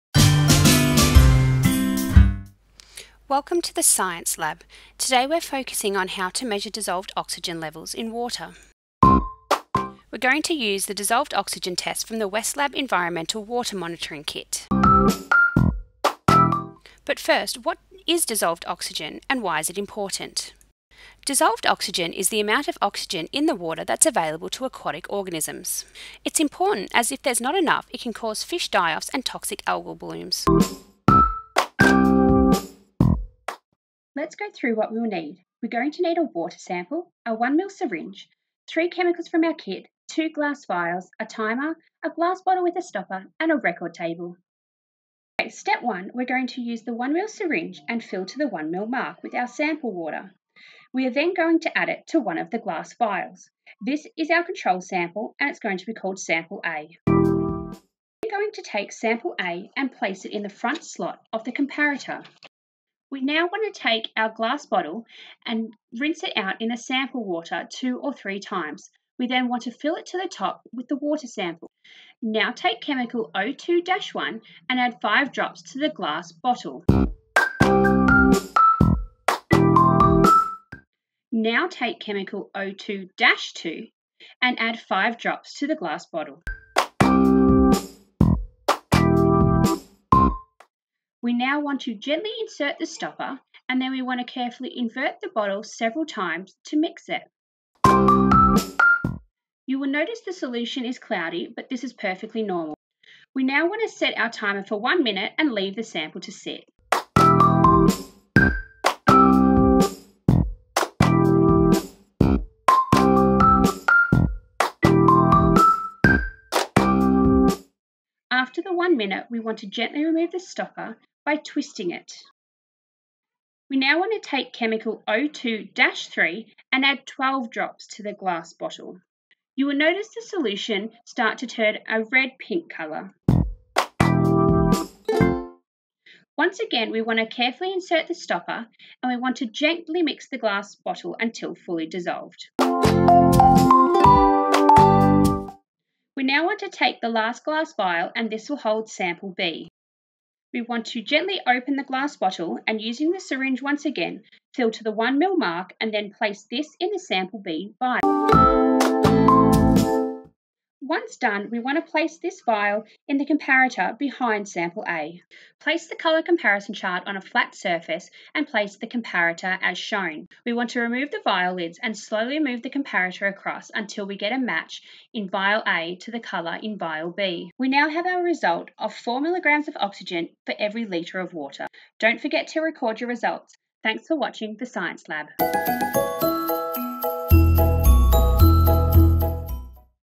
Measuring Dissolved Oxygen levels in Water
Using a modified Winkler method to test DO levels in water samples. Kit is from the Westlab Environmental Water management kit.
Shows step by step, easy to follow instructions for students to follow along and complete themselves.